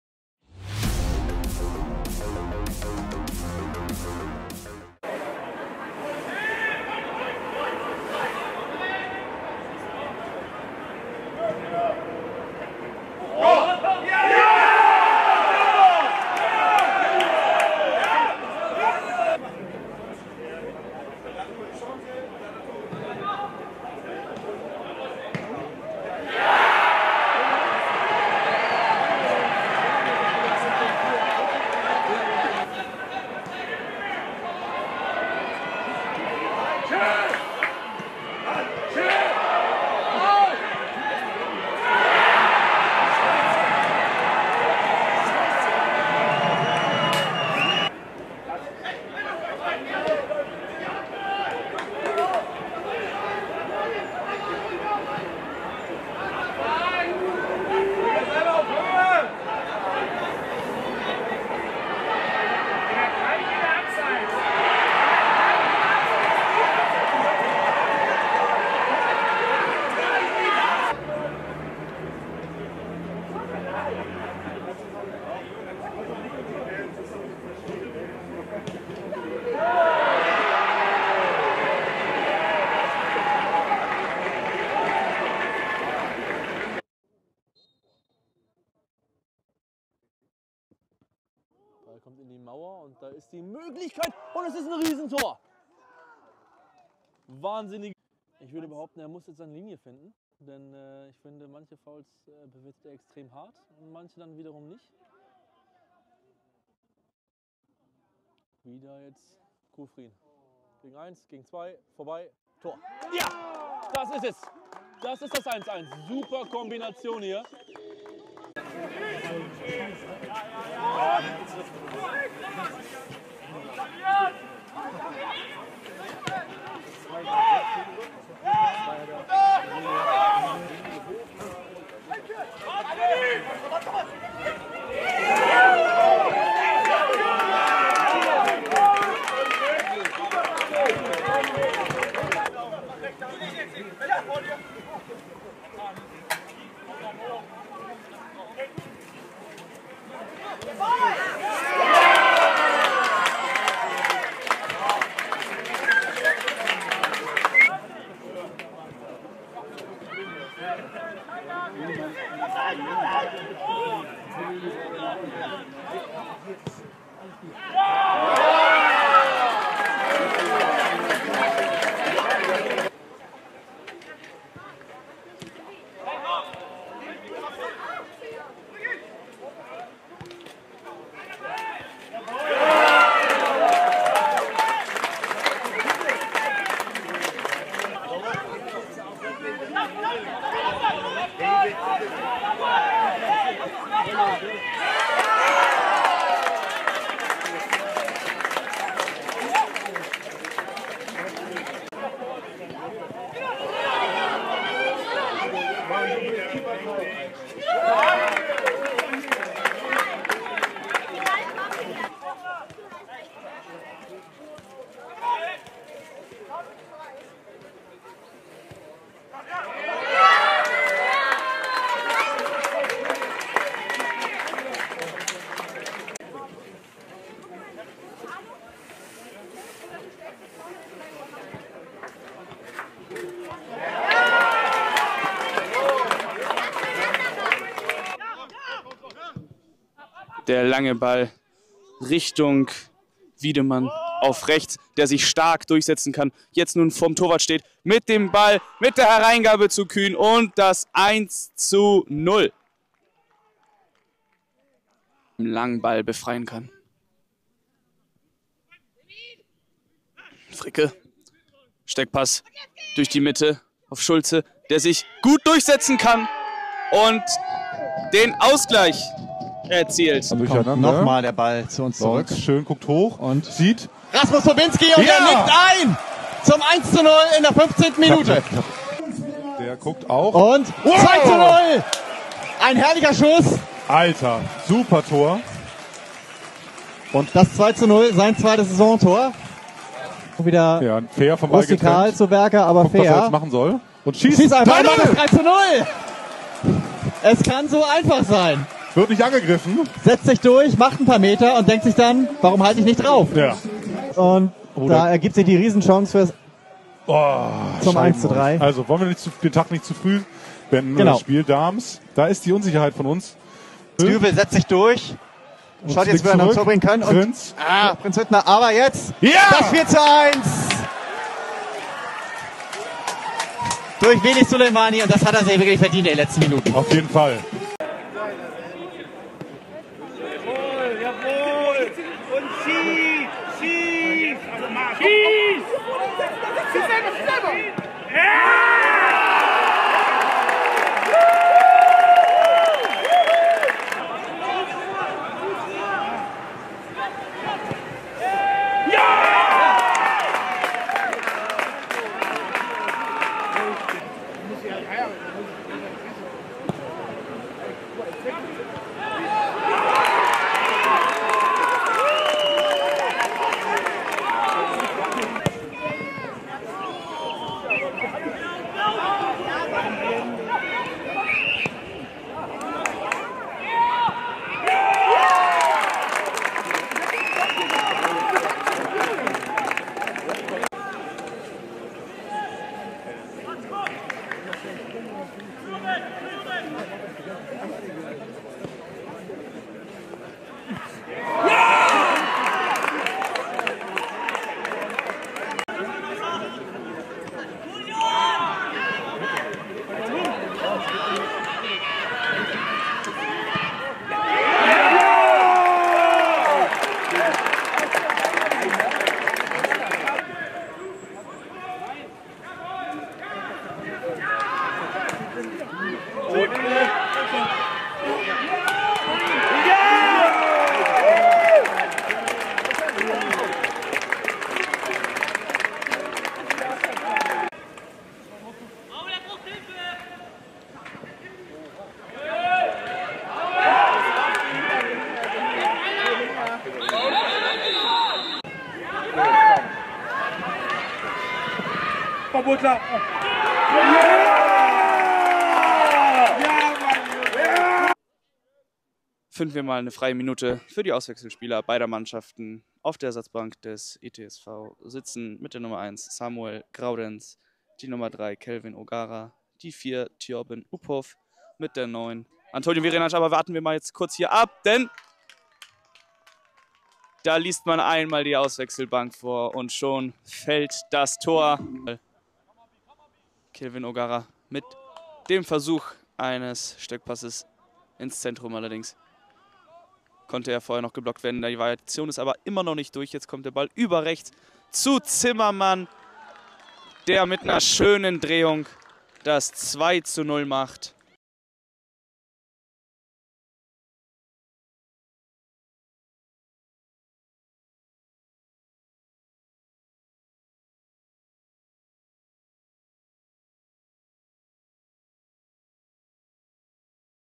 Torshow 13. Spieltags Gamesright Oberliga Hamburg
Alle Tore des 13. Spieltages der Gamesright Oberliga Hamburg.

Zu mehr Infos rund um die Gamesright Oberliga Hamburg hier entlang